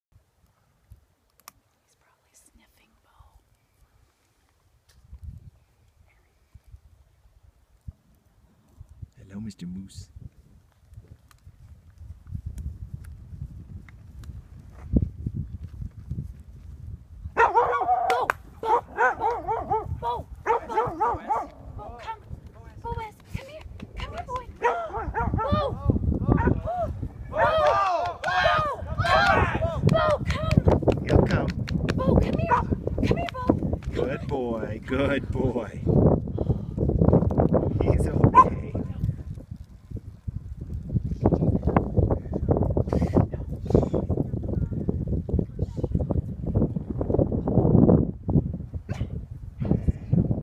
Golden Doodle Moose Encounter in Bighorns
A fun encounter with a big moose in the Bighorn mountains at the tail waters of Grace Lake.